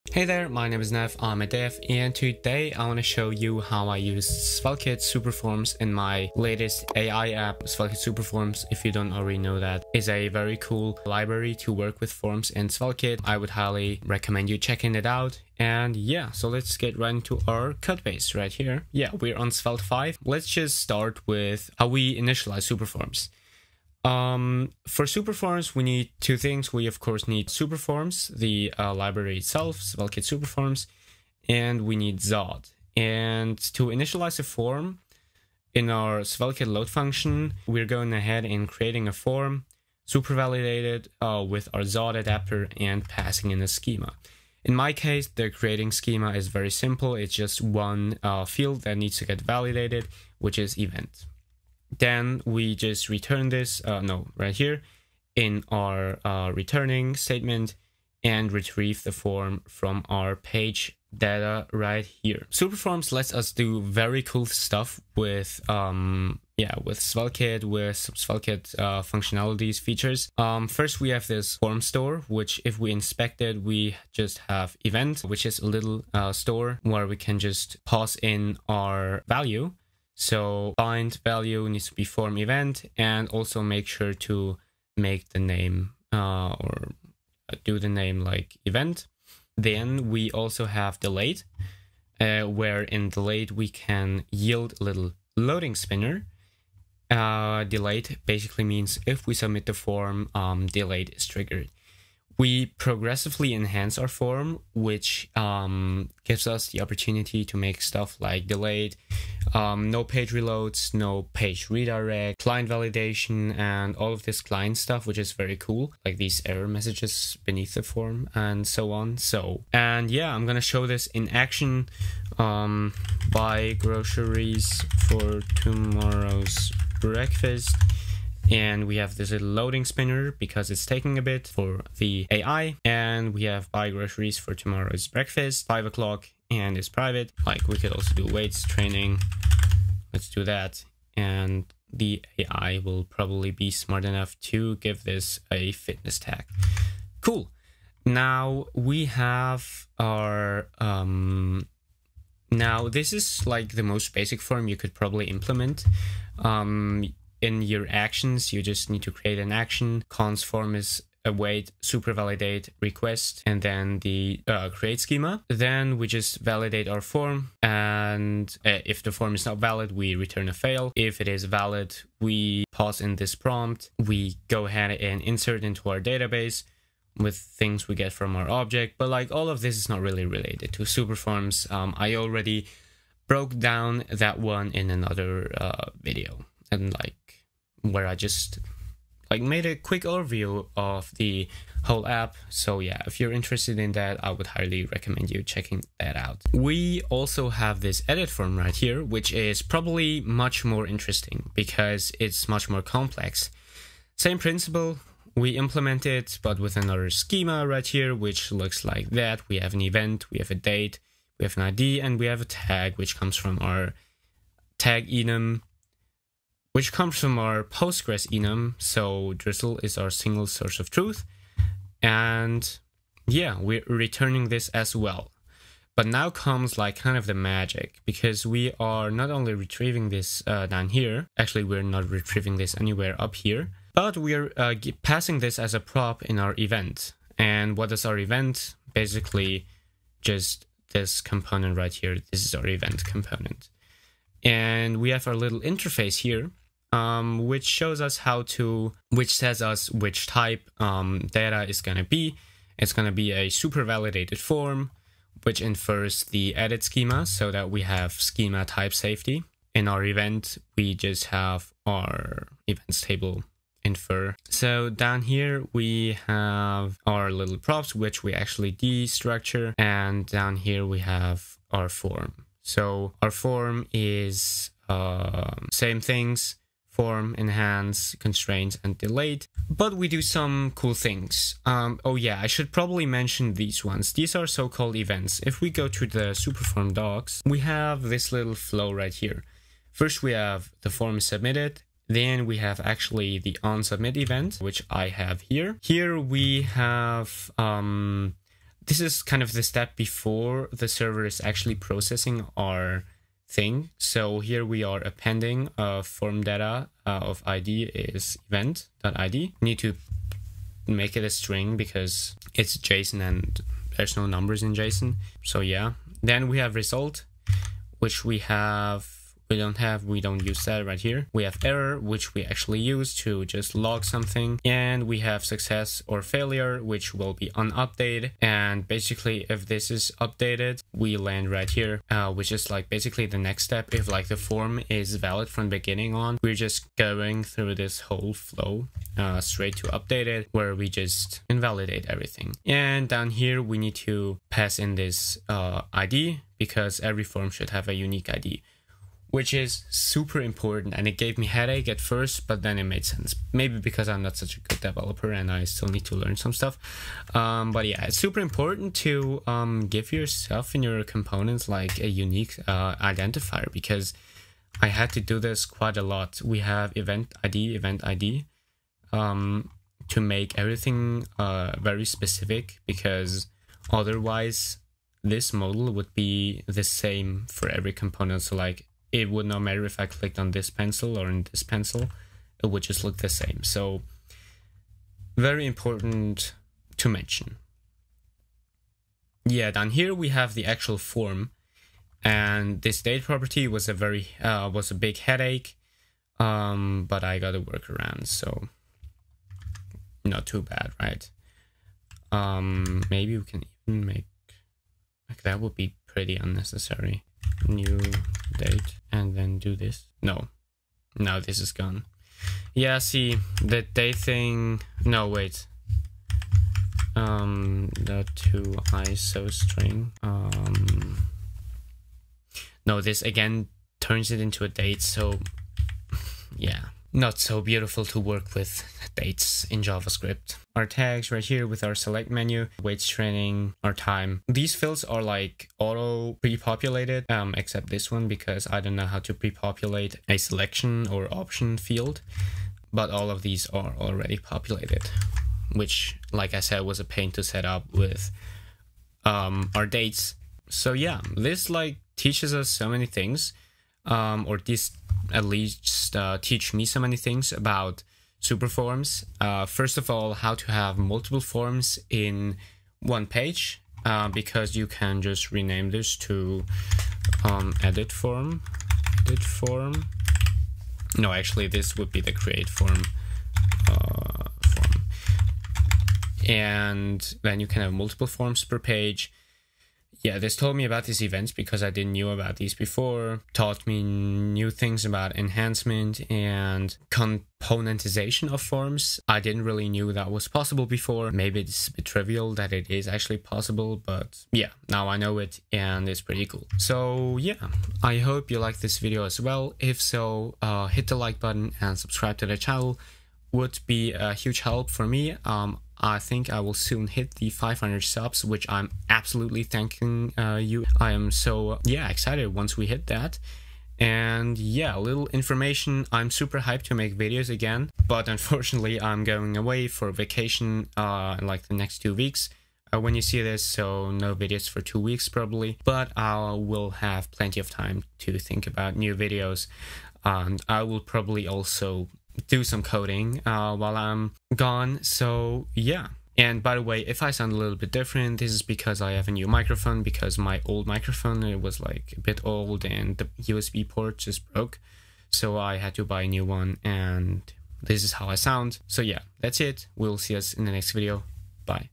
advanced forms with SvelteKit Superforms
00:00 Intro
00:17 Initializing the form
01:02 Retrieving the form
01:29 Loading spinner
01:59 Form showcase
02:32 Server Side
03:16 Multiple forms per page
04:43 Events
06:29 Date type pain
07:23 Takeaways
08:17 Outro